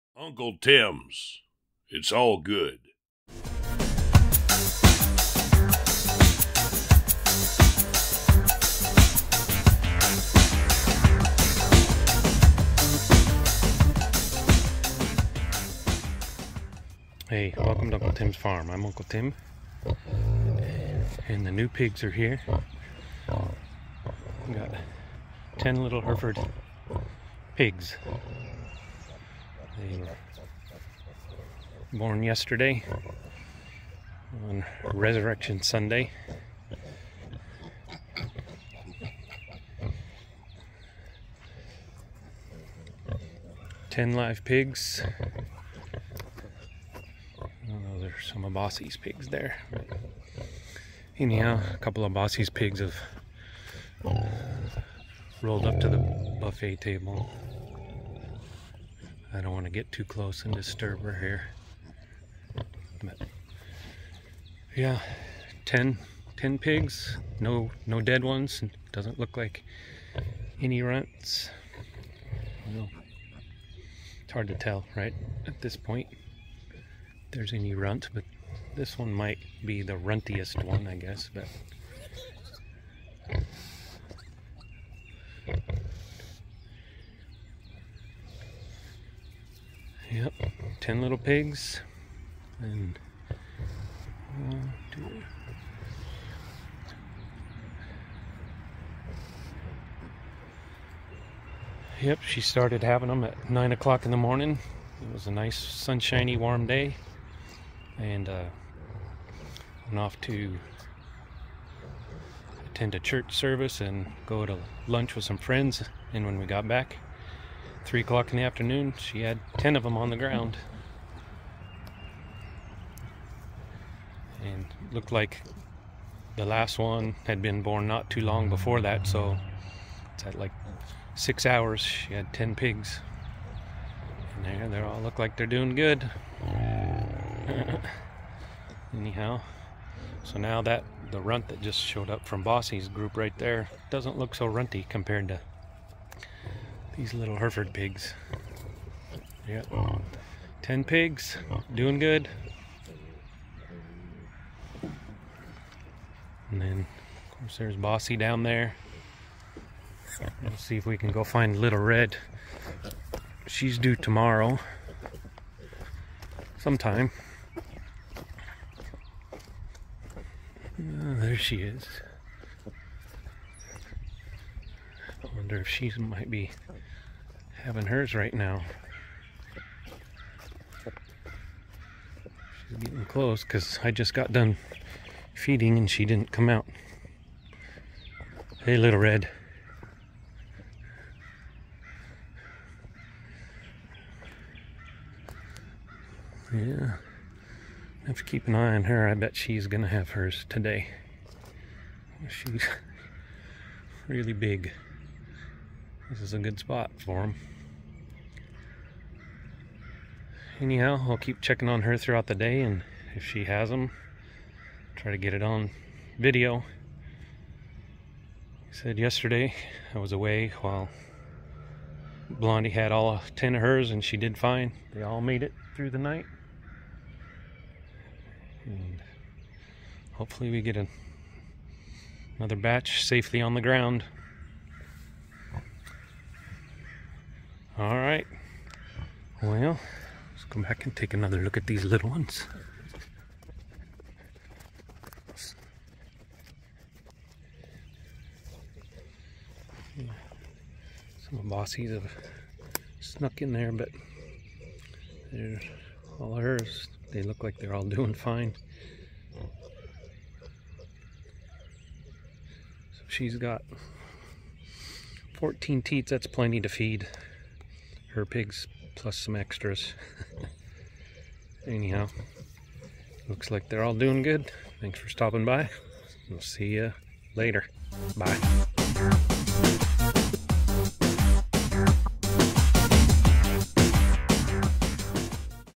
Hereford Piglets Finally Born! But How Many?
Blondie has finally farrowed her Hereford piglets.  Get a look at them within minutes of them being born and also how they are doing the next day.